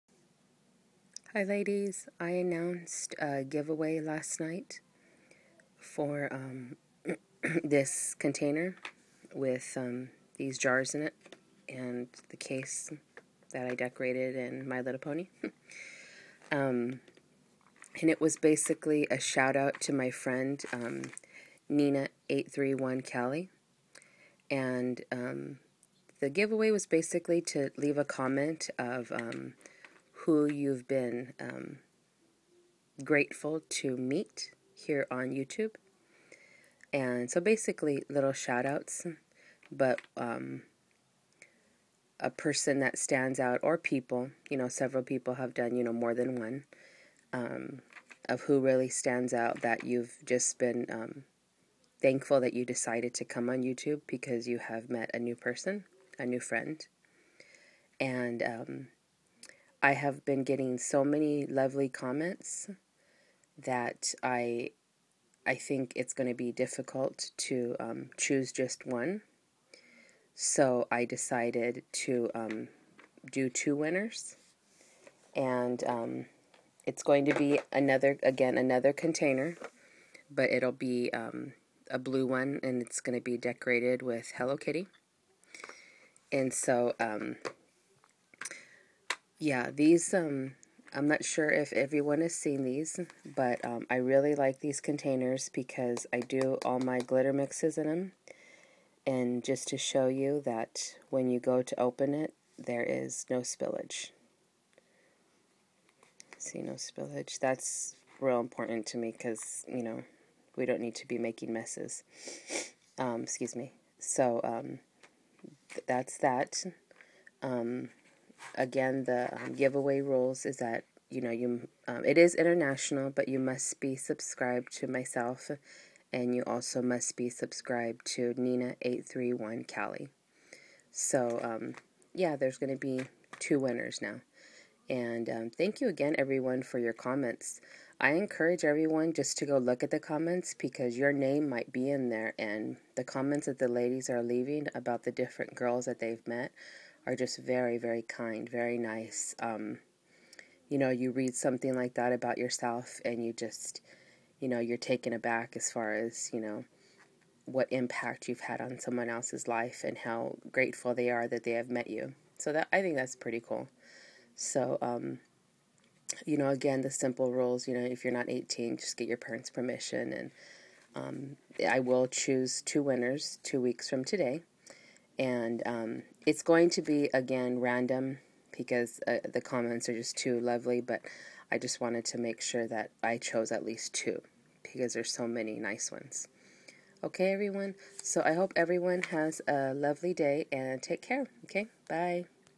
CLOSED***GIVEAWAY PRIZE ADDED
So many lovely comments had to add another prize. So now there will be two winners.
1. Must be subbed to nina831cali
2. Must be subbed to me.
3. If you are not 18, get your parents permission.
4. This is international